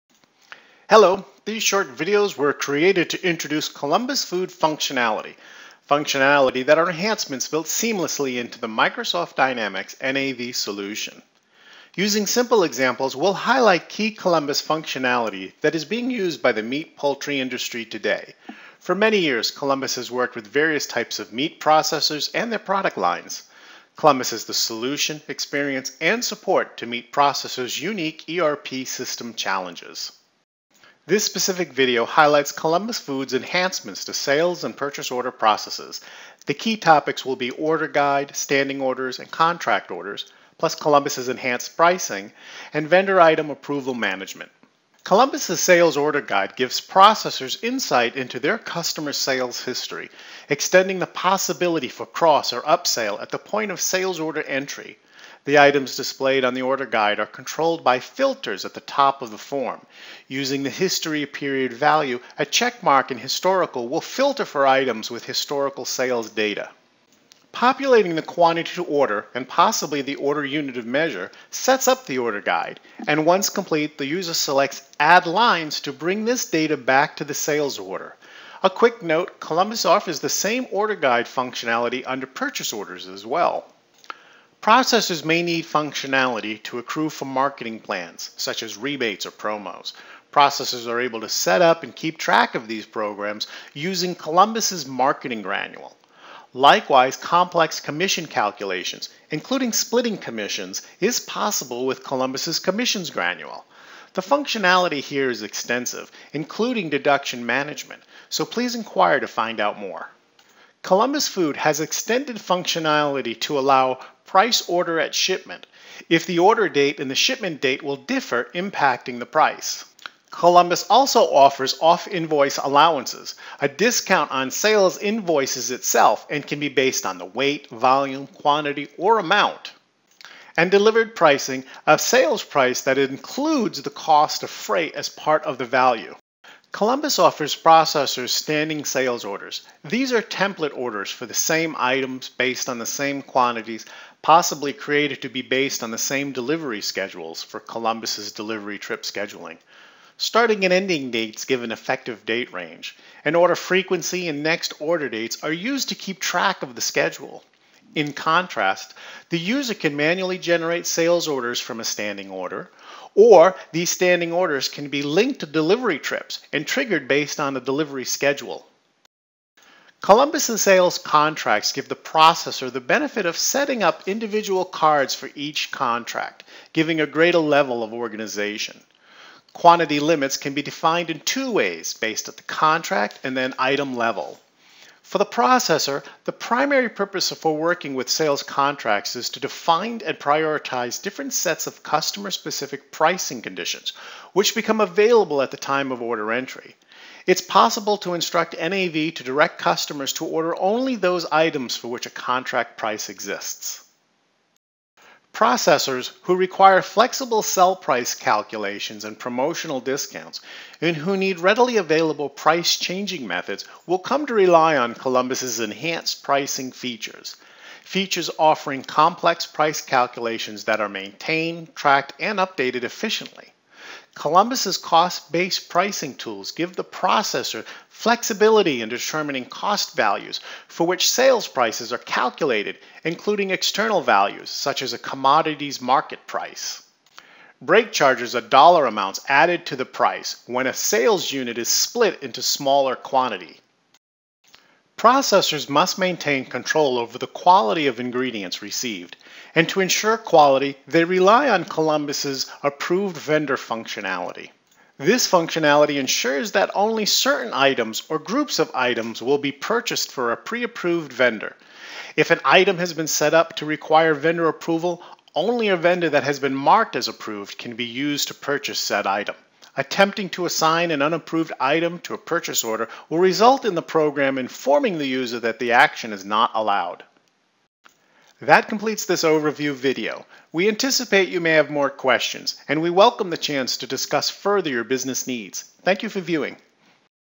ColumbusFood ERP for Meat & Poultry - Sales Order Processing
Columbus Food's key functionality built in to Microsoft Dynamics NAV specifically for companies in the Meat & Poultry industry. This short demo highlights Columbus Food's Recipe Management functionality.